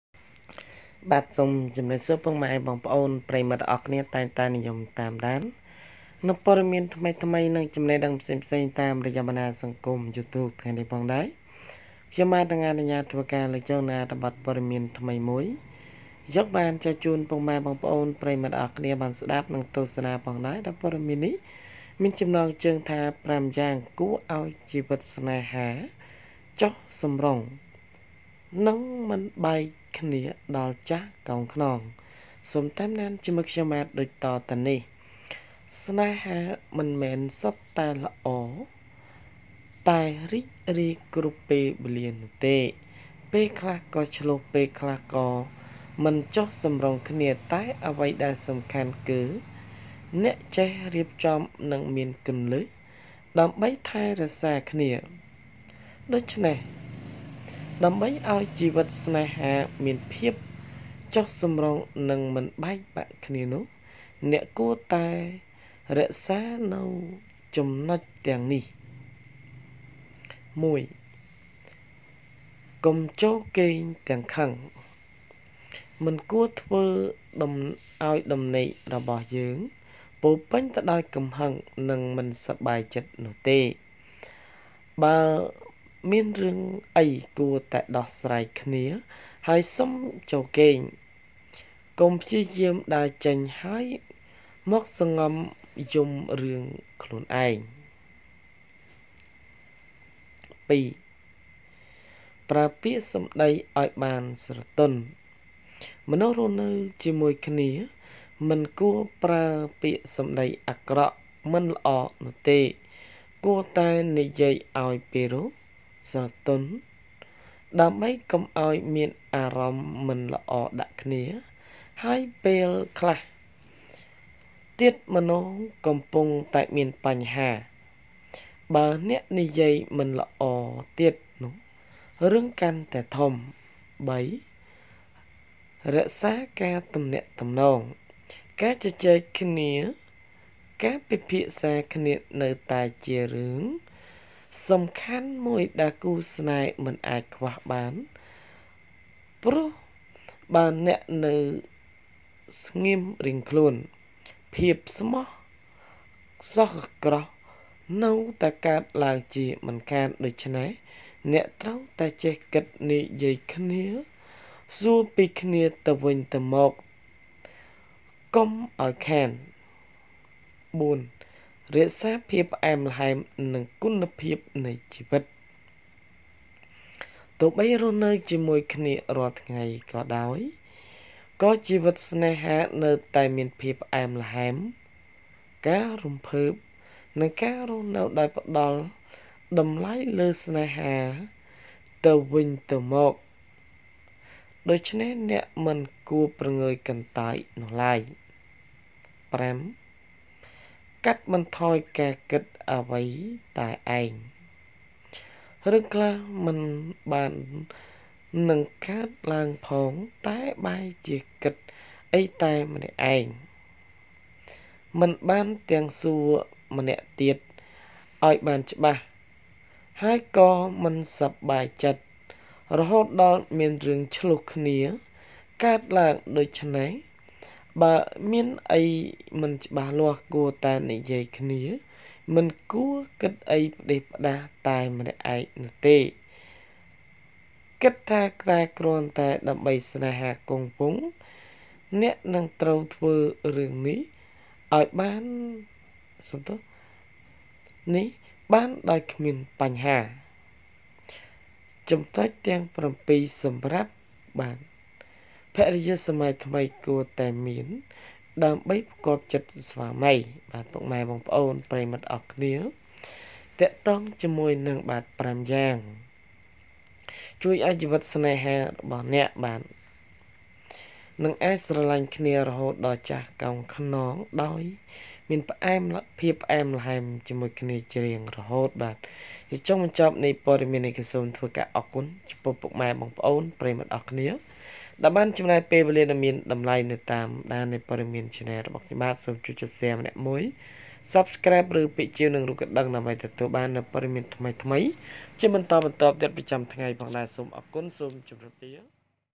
៥ យ៉ាងជួយឲ្យ​ជីវិត​ស្នេហា​​ចុះ​សម្រុង និង​មិន​បែក​គ្នា​ដល់​ចាស់​កោងខ្នង
Five things help keep your love life calm and unbroken

សូមស្វាគមន៍មកកាន់បណ្តាញព័ត៌មានសូមកុំភ្លេចចុះឈ្មោះជាវប៉ុស្តិ៍របស់ខ្ញុំសូមអរគុណ!

Cambodia Freedom

សូមអរគុណយ៉ាងជ្រាវជ្រៅវ
Thank You So much !